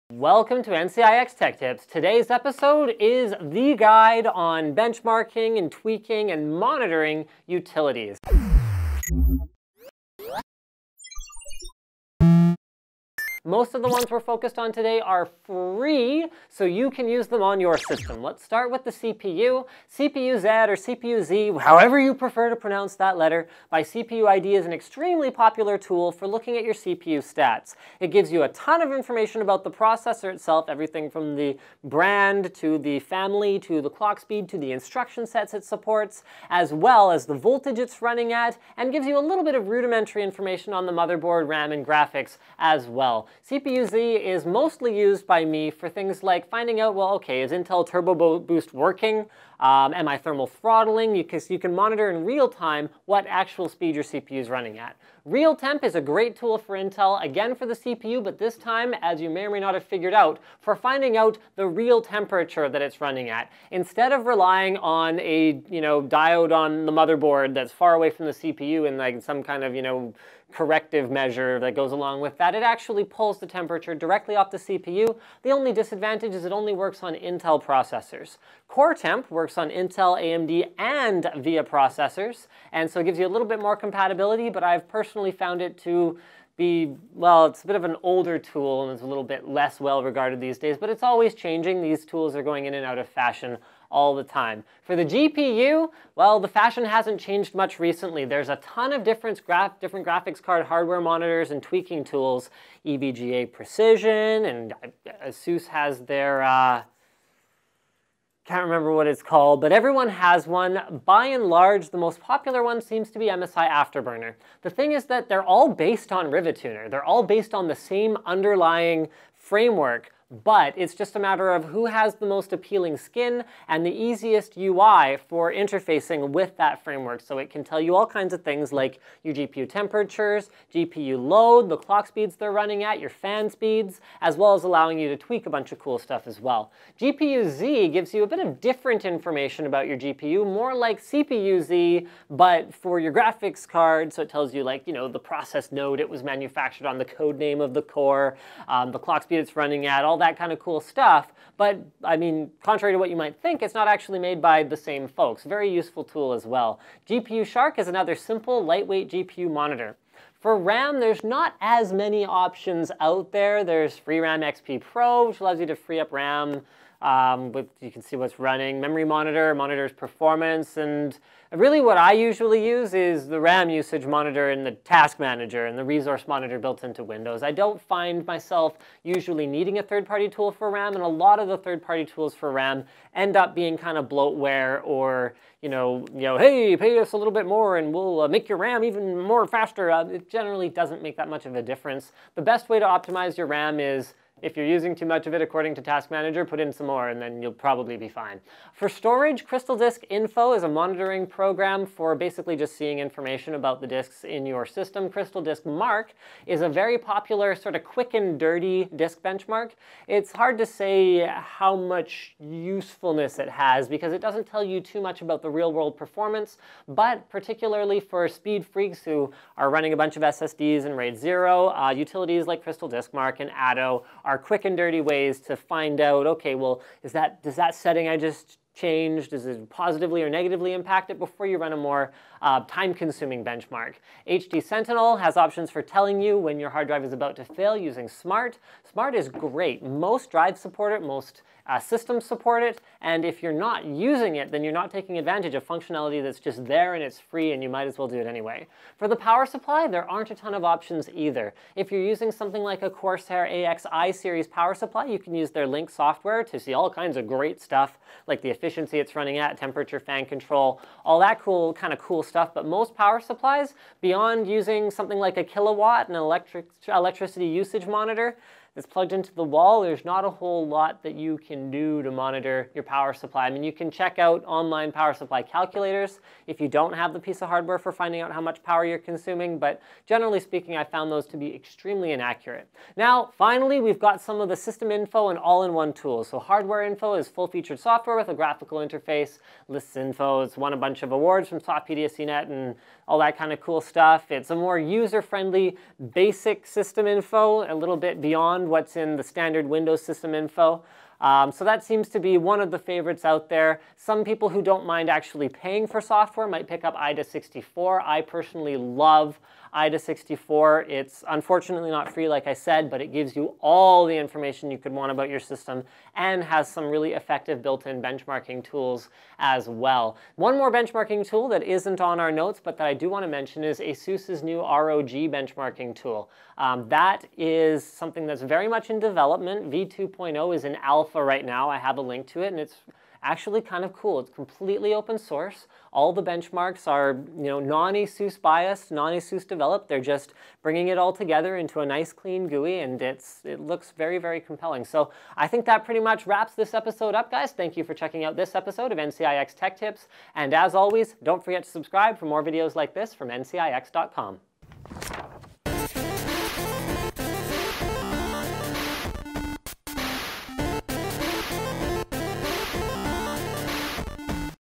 Hardware Monitoring/Tweaking Software Guide
Keeping tabs on all your computer components is tough.  This guide shows you some simple ways of making sure your computer doesn't fail spectacularly.  Temperatures can be monitored for almost all of your components with various pieces of software.

Task Manager - Built In